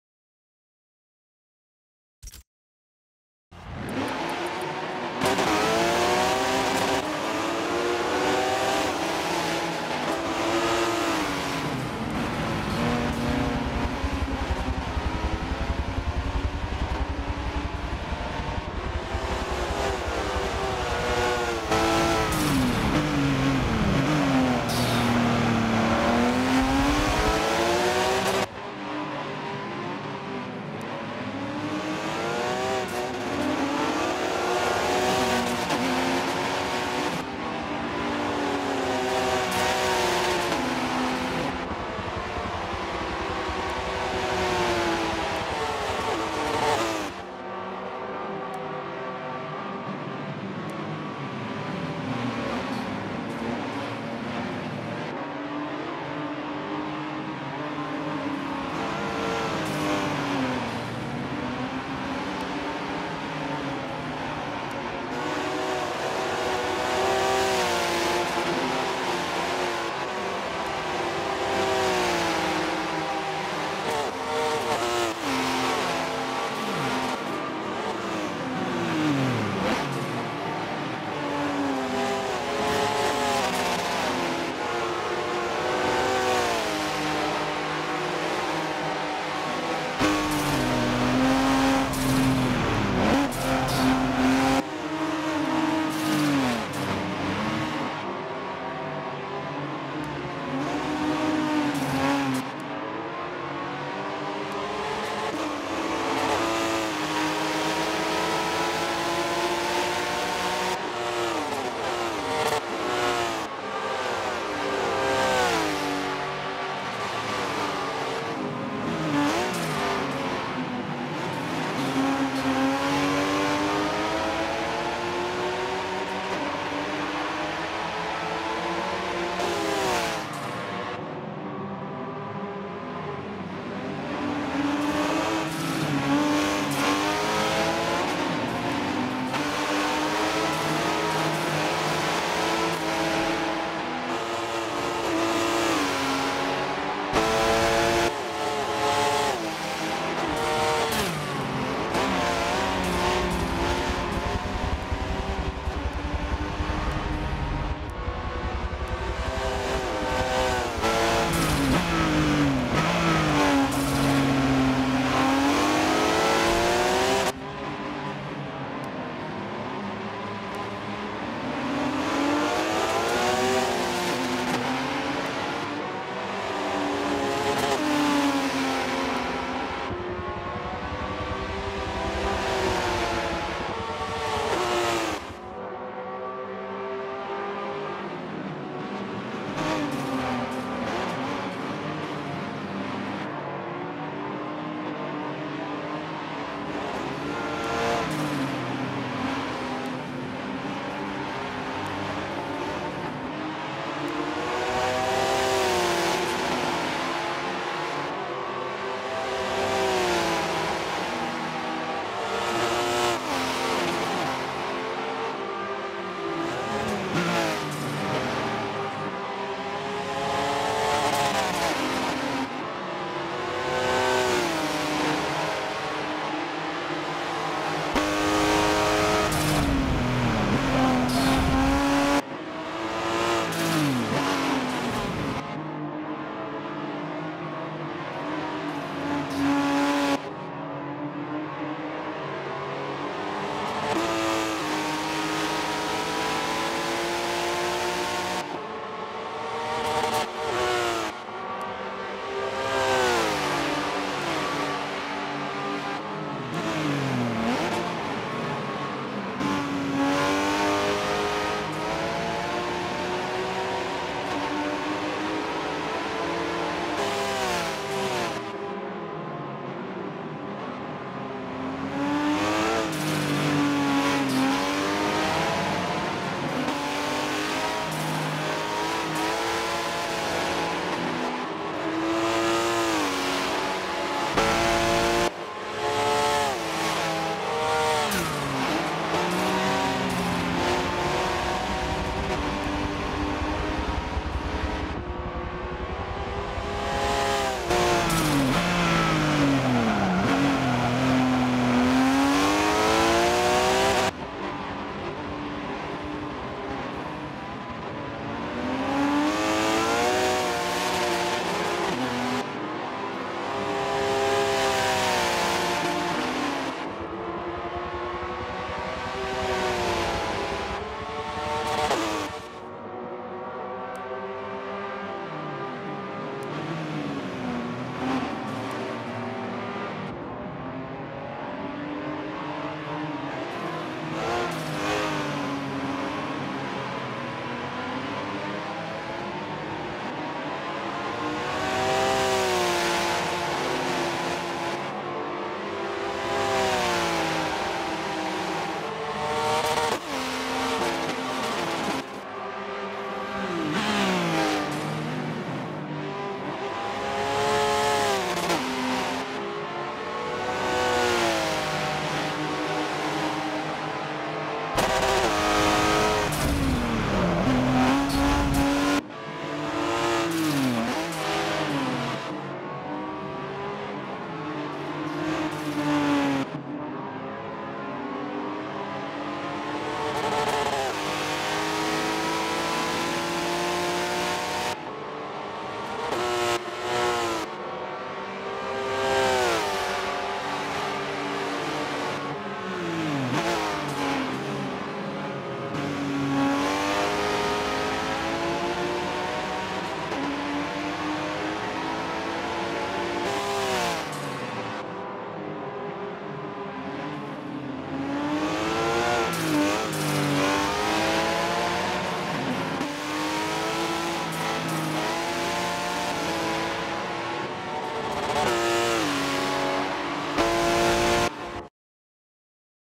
7️⃣ "Porsche 959 (1987) Cup" sur Silverstone
Course en "Porsche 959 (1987) Cup" sur Silverstone